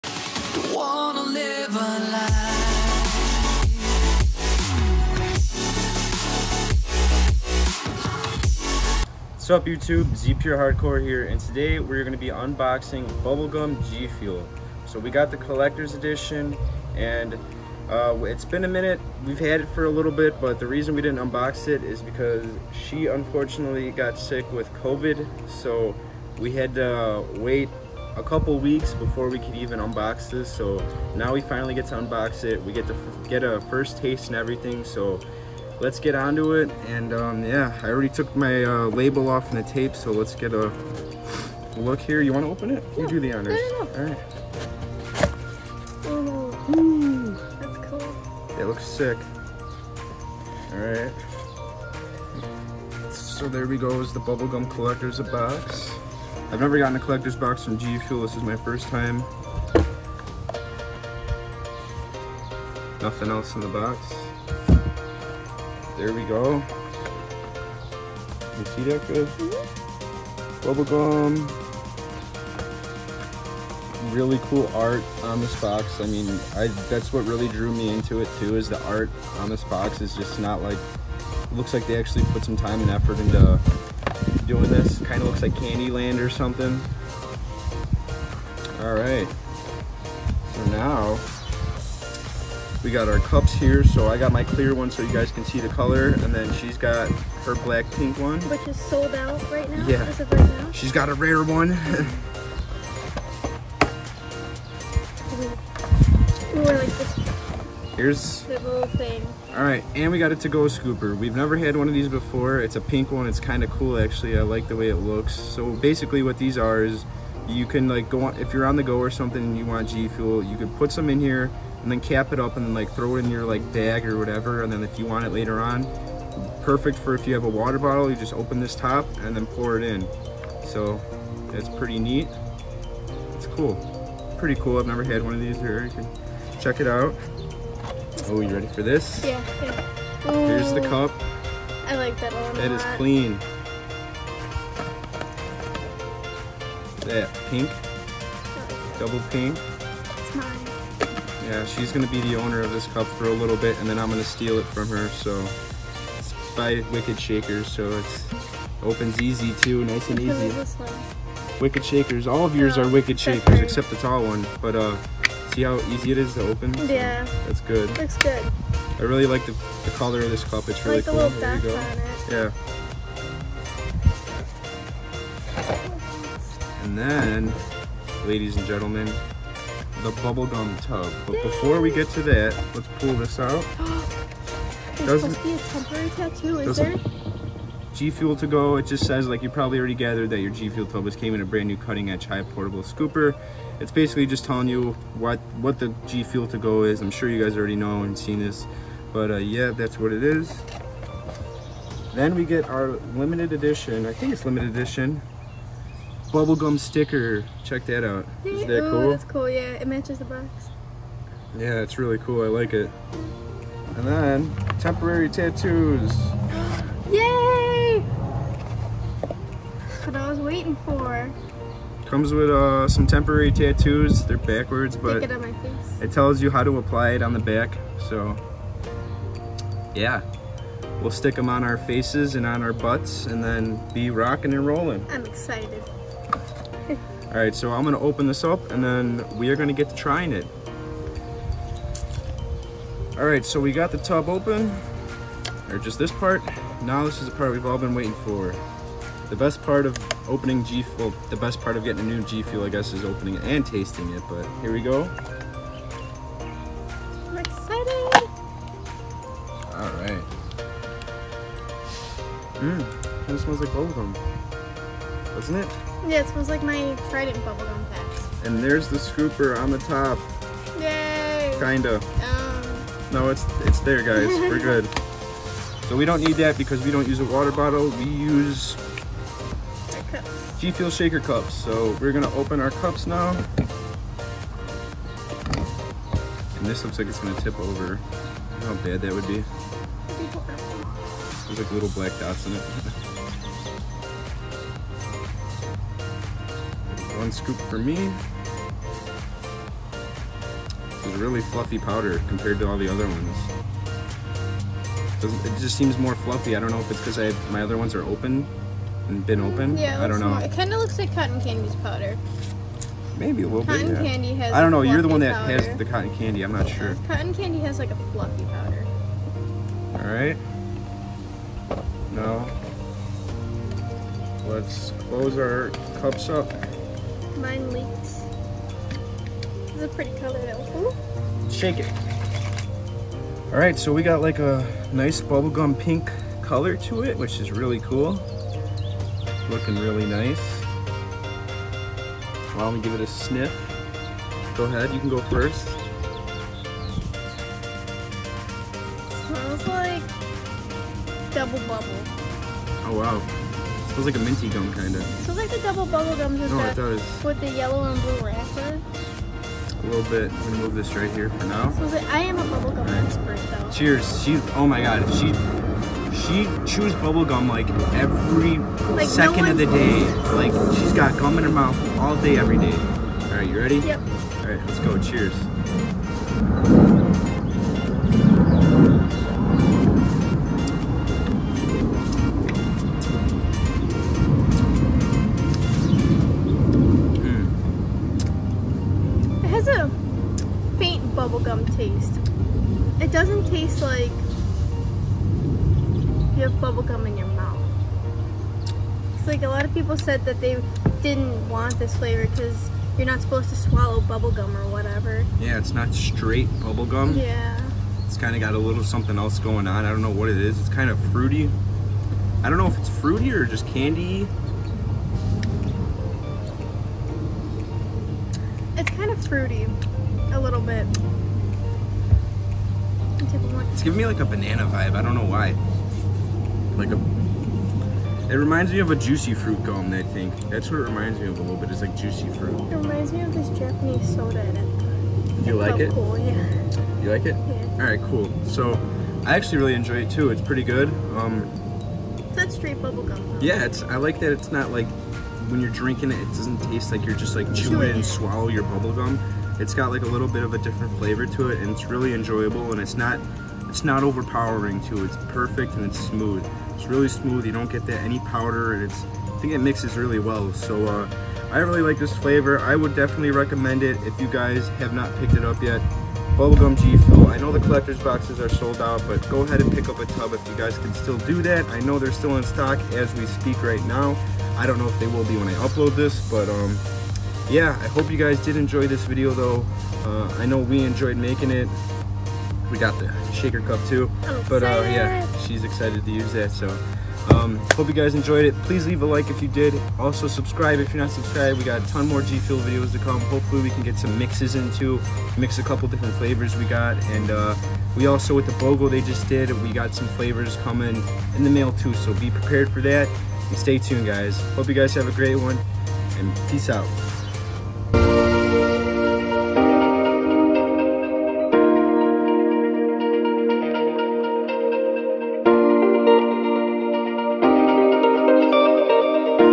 Bubble Gum GFUEL Unboxing and Taste Test!!
Sup youtube! Today we try the new Bubble Gum GFUEL for the first time!! Hope you guys enjoy! :D Have a awesome day!
|SOCIALS|